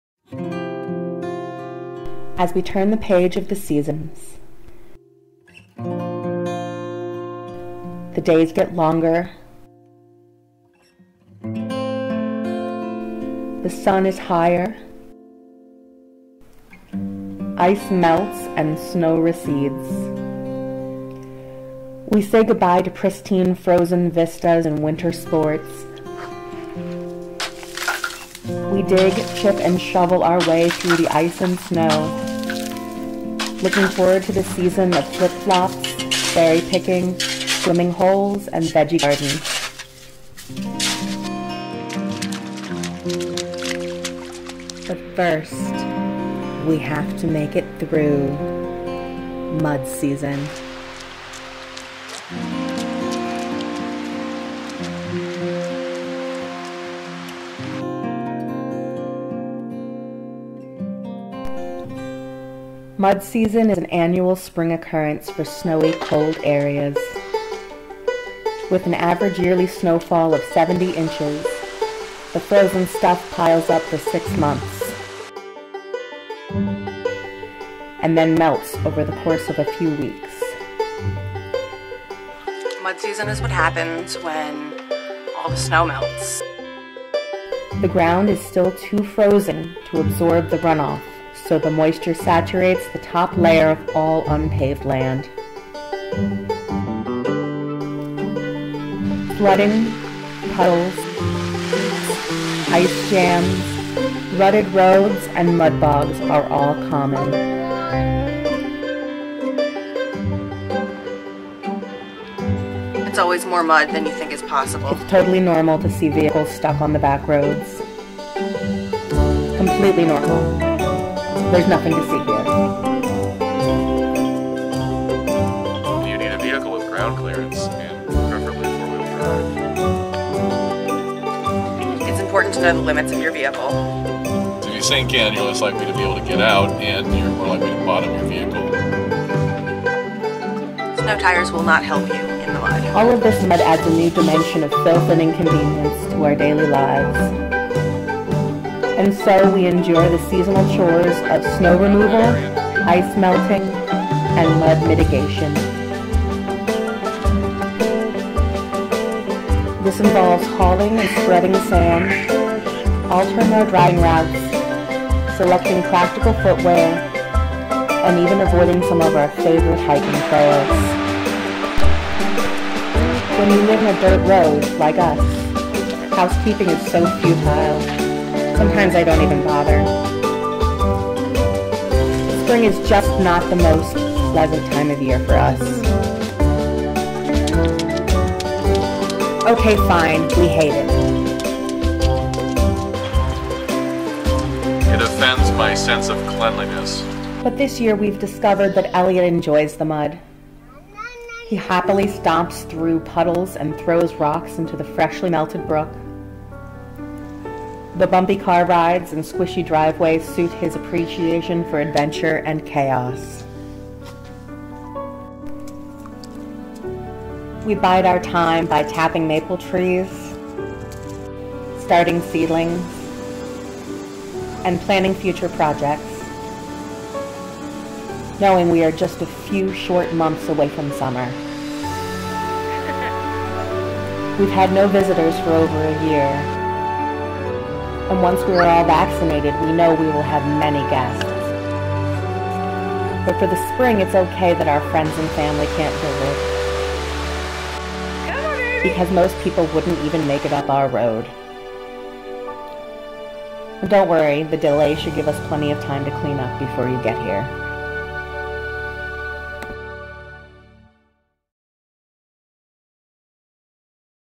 Story 3-Mud Season
Experience Mud Season in the Green Mountains. My family deals with the chores and frustration of spring. The volume and intensity of the mud can't be explained. Just watch.